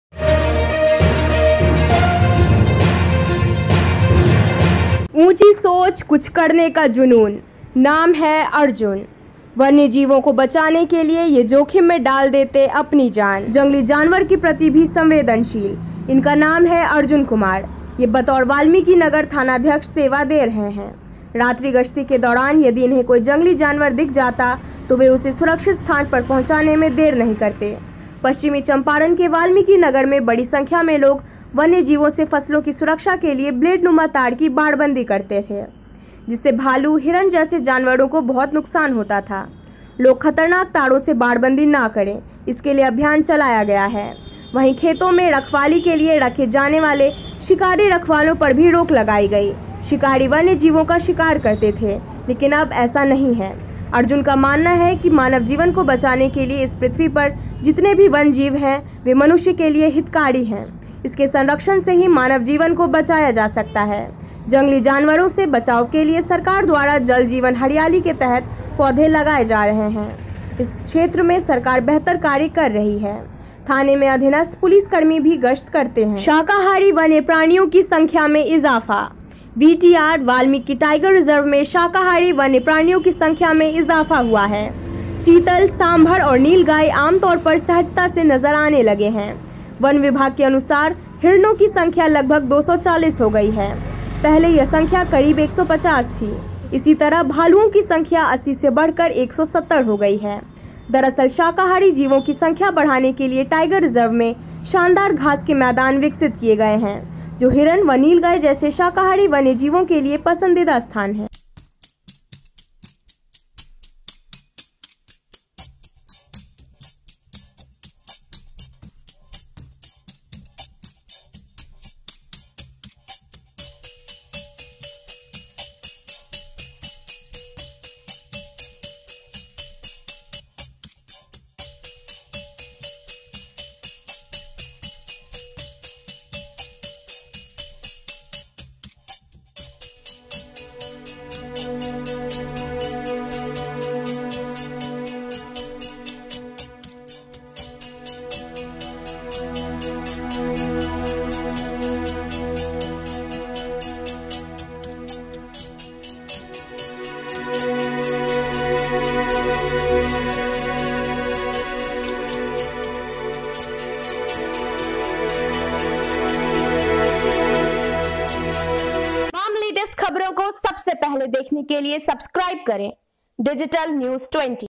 ऊंची सोच, कुछ करने का जुनून, नाम है अर्जुन
DN20, Bagha : ऊंची सोच और कुछ करने का माद्दा। जंगली जानवर के प्रति भी संवेदनशील। इनका नाम है-अर्जुन कुमार। ये बतौर वाल्मीकिनगर थानाध्यक्ष सेवा दे रहे हैं। रात्रि गश्ती के दौरान यदि इन्हें कोई जंगली जानवर दिख जाता तो वे उसे सुरक्षित स्थान पर पहुंचाने में देर नहीं करते।
पश्चिमी चंपारण के वाल्मीकिनगर में बड़ी संख्या में लोग वन्य जीवों से फसलों की सुरक्षा के लिए ब्लेडनुमा तार की बाड़बंदी करते थे। जिससे भालू, हिरण जैसे जानवरों को बहुत नुकसान होता था। लोग खतरनाक तारों से बाड़बंदी न करें, इसके लिए अभियान चलाया गया है। वहीं, खेतों में रखवाली के लिए रखे जानेवाले शिकारी रखवालों पर भी रोक लगाई गई। शिकारी वन्य जीवों का शिकार करते थे, लेकिन अब ऐसा नहीं है। अर्जुन का मानना है कि मानव जीवन को बचाने के लिए इस पृथ्वी पर जितने भी वन जीव हैं, वे मनुष्य के लिए हितकारी हैं। इसके संरक्षण से ही मानव जीवन को बचाया जा सकता है। जंगली जानवरों से बचाव के लिए सरकार द्वारा जल जीवन हरियाली के तहत पौधे लगाए जा रहे हैं। इस क्षेत्र में सरकार बेहतर कार्य कर रही है। थाने में अधीनस्थ पुलिसकर्मी भी गश्त करते हैं।
शाकाहारी वन्य प्राणियों की संख्या में इजाफा
वीटीआर (वाल्मीकि टाइगर रिजर्व) में शाकाहारी वन्य प्राणियों की संख्या में इजाफा हुआ है। चीतल, सांभर और नीलगाय आमतौर पर सहजता से नजर आने लगे हैं। वन विभाग के अनुसार हिरणों की संख्या लगभग 240 हो गई है। पहले यह संख्या करीब 150 थी। इसी तरह भालुओं की संख्या 80 से बढ़कर 170 हो गई है। दरअसल, शाकाहारी जीवों की संख्या बढ़ाने के लिए टाइगर रिजर्व में शानदार घास के मैदान विकसित किए गए हैं। जो हिरण व नीलगाय जैसे शाकाहारी वन्य जीवों के लिए पसंदीदा स्थान हैं।